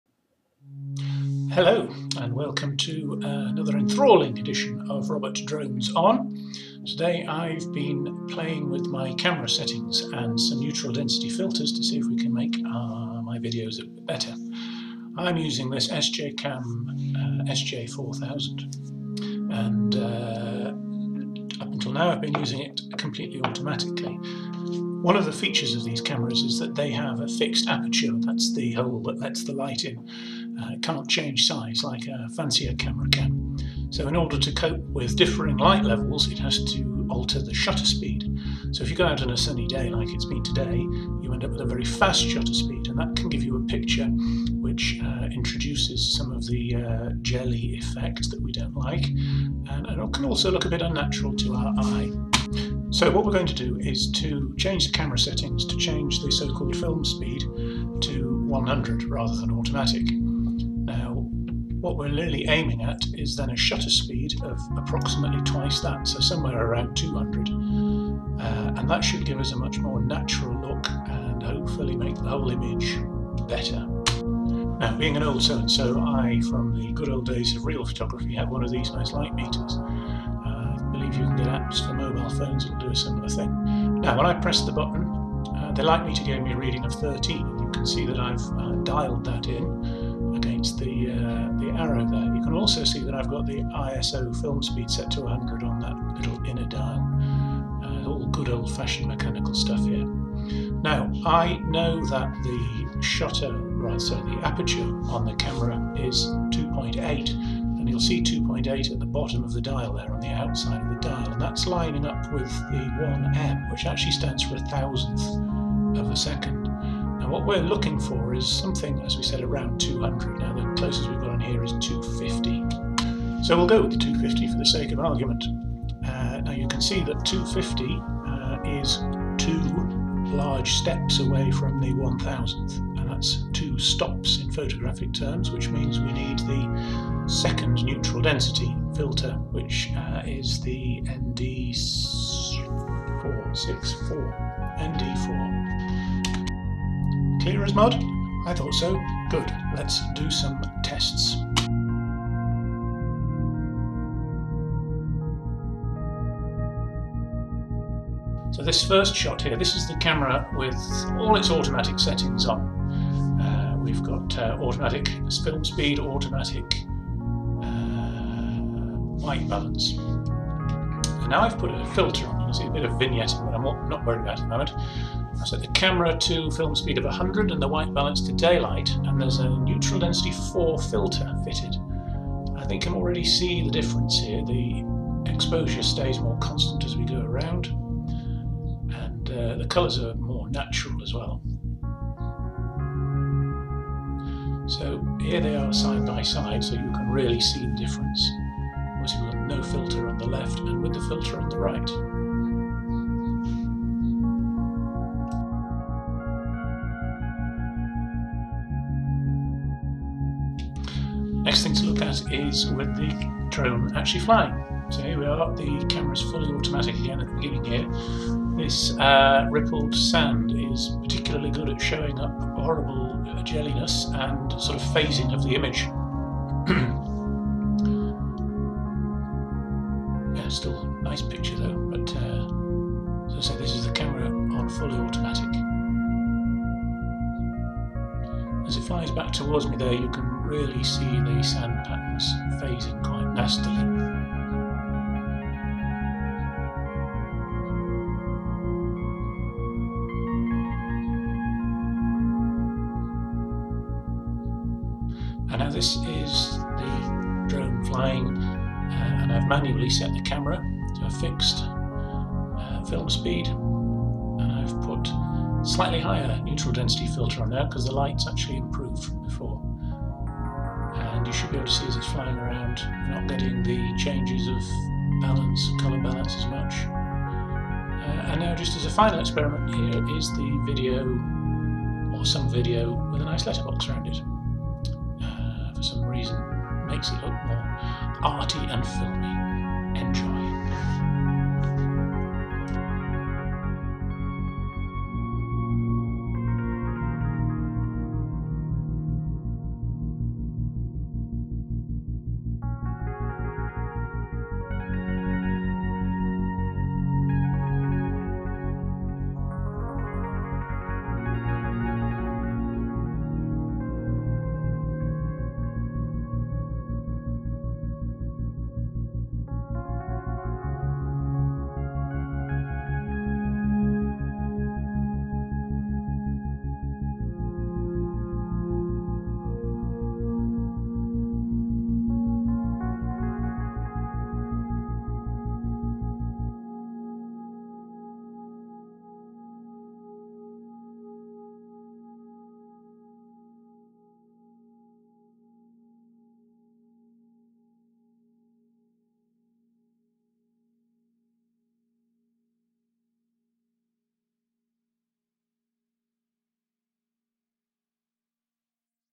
Robert Drones On: ND Filter Test
Testing the use of neutral density filters on my drone's camera as a means of improving video quality.